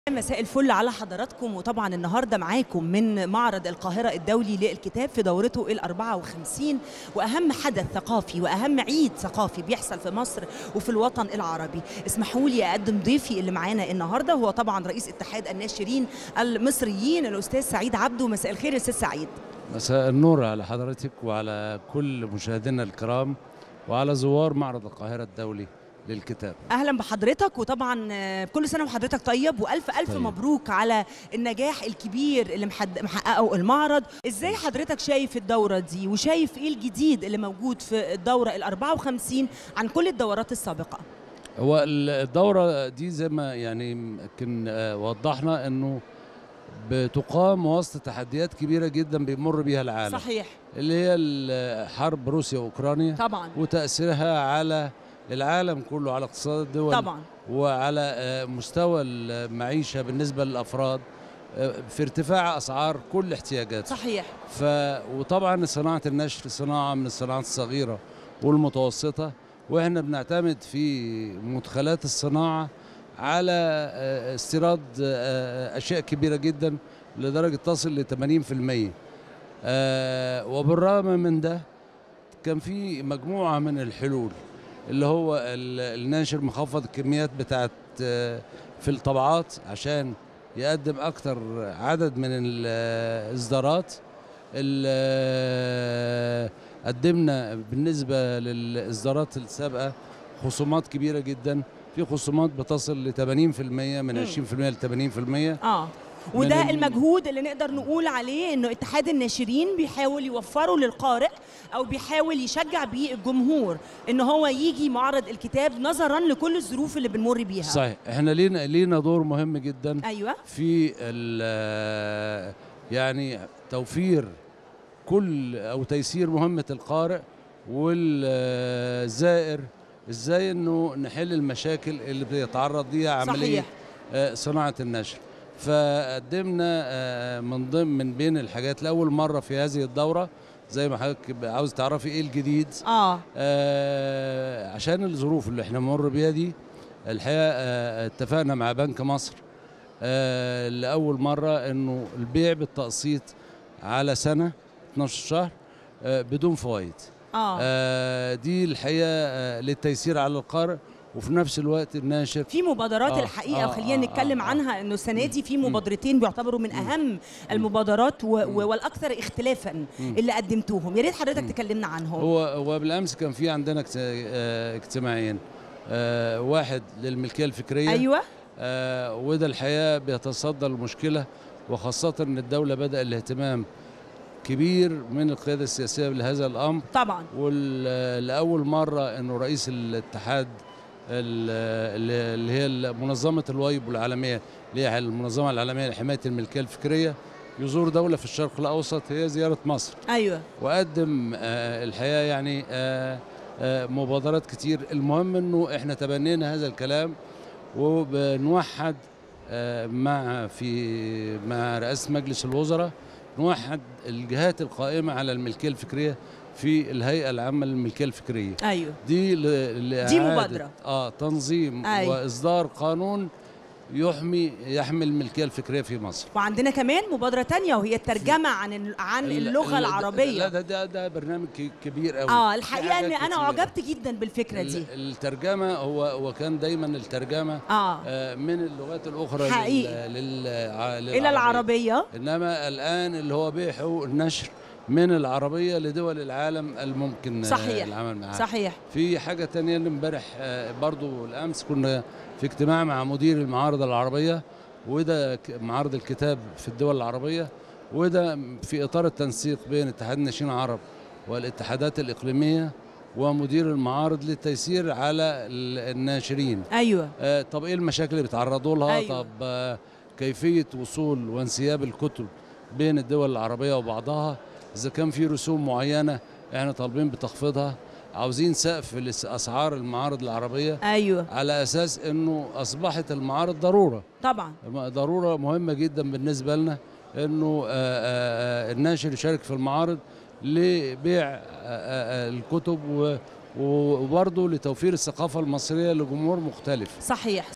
رئيس اتحاد الناشرين:هناك اتفاق لأول مرةمع بنك مصري للشراءبالتقسيط وهناك كتب يصل الخصم فيها إلى 80 ٪
يحكي أن | رئيس اتحاد الناشرين المصريين: هناك اتفاق لأول مرة مع بنك مصري لشراء الكتب بالتقسيط وهناك كتب يصل الخصم فيها إلى 80 ٪